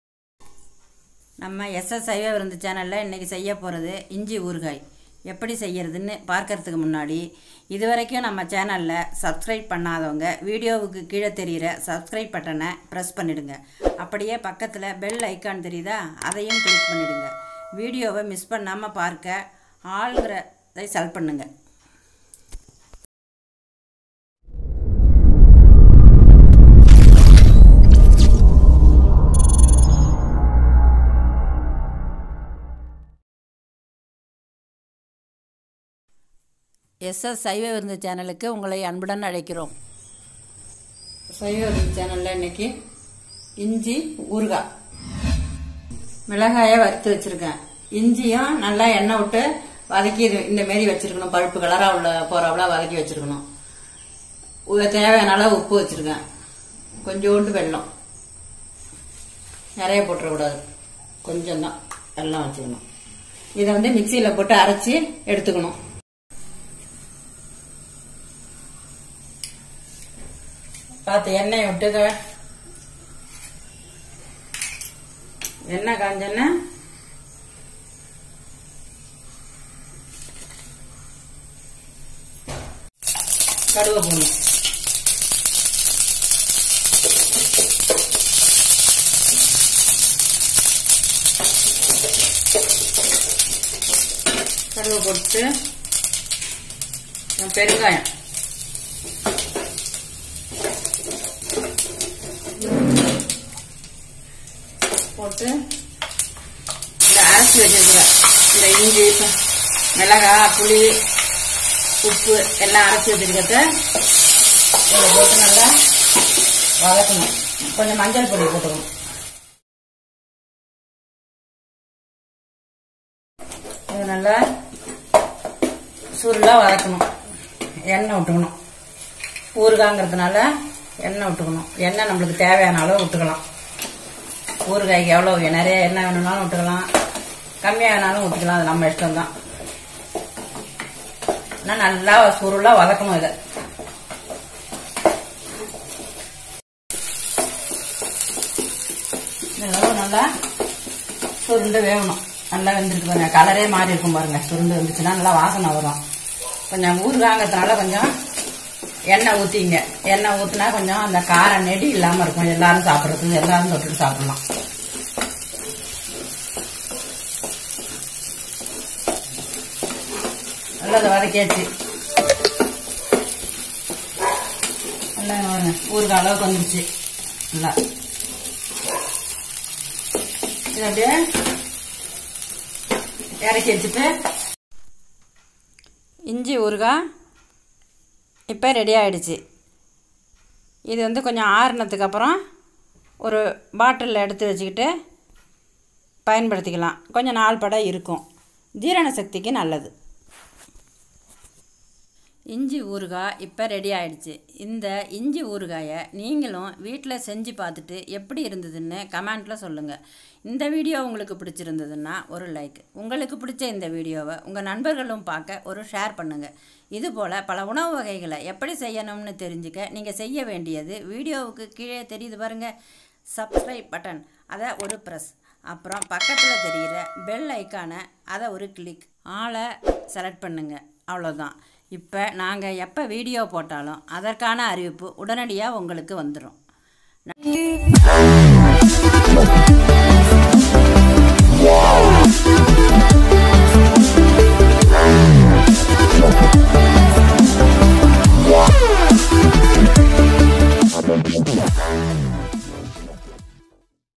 Ginger Pickle Making in Tamil| சுவையான இஞ்சி ஊறுகாய் செய்வது எப்படி தமிழில் | SS Saiva Virunthu
In this video, We explained in practically making Ginger Pickle in Tamil Language.

If you like this video Please give thumbs up and share to your friends and Colleagues.

சுவையான இஞ்சி ஊறுகாய் செய்வது எப்படி என்பது பற்றி இக்காணொளியில் எளிமையாக பதிவிடப்பட்டுள்ளது .

பார்த்து பயன் பெறுங்கள்

நீங்கள் பார்த்து பயன்பெற்ற இந்த வீடியோவை உங்கள் நண்பர்களும் பார்த்து பயன் பெற ஒரு ஷேர் பண்ணுங்க